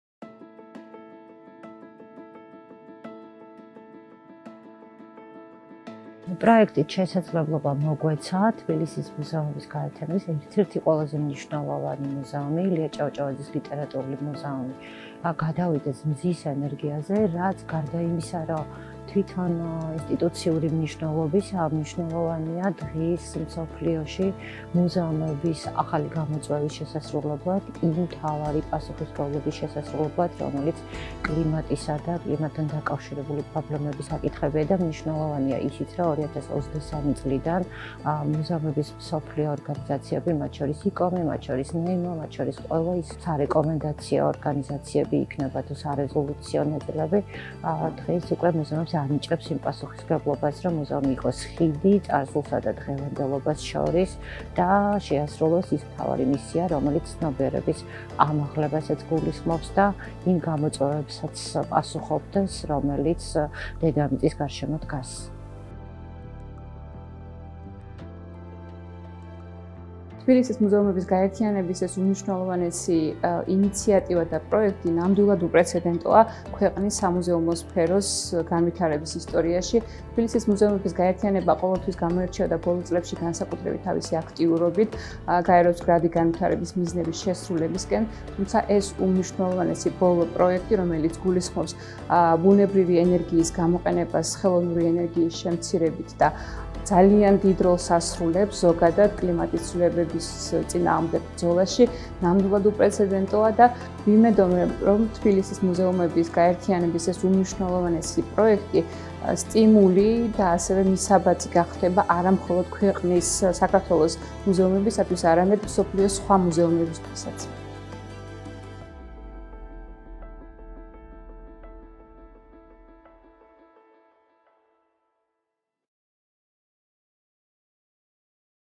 ილია ჭავჭავაძის ლიტერატურულ - მემორიალურ მუზეუმში მზის პანელების სისტემა მოეწყო
bp-ის მიერ დაფინანსებული პროექტის „მეტი ინოვაცია და საზოგადოებრივი ჩართულობა თბილისის მუზეუმების გაერთიანებაში“  - ილია ჭავჭავაძის ლიტერატურულ - მემორიალურ მუზეუმში მზის პანელების სისტემა მოეწყო.
ეს არის პირველი შემთხვევა ამიერკავკასიაში, როცა მუზეუმი ბუნებრივი ენერგიის წყაროზე გადავიდა, შეამცირა გარემოზე ნეგატიური ზეგავლენა და მზის ენერგიას მოიხმარს.სიახლე დაზოგავს მინიმუმ 30 000 კილოვატ ტრადიციულ ელექტროენერგიას, რაც  საშუალებას მისცემს მუზეუმს დაზოგოს ელექტროენერგიის გადასახადი,  და 20 წლის განმავლობაში ხელს შეუწყობს  დაახლოებით 150 ტონა CO2 ემისიის დაკავებას და გადაარჩენს 8000-მდე ხეს.
თბილისის მუზეუმების გაერთიანება პირველი სამუზეუმი ინსტიტუციაა ამიერკავკასიაში, რომელიც ბუნებრივი ენერგიის წყაროზე გადავიდა და ჩაერთო გარემოს დაცვის საერთო კამპანიაში.
პროექტი ხორციელდება კომპანია bp-ის დაფინანსებით, კონსულტაციისა და ტრენინგის ცენტრის (CTC) და თბილისის მუზეუმების გაერთიანების თანამშრომლობით.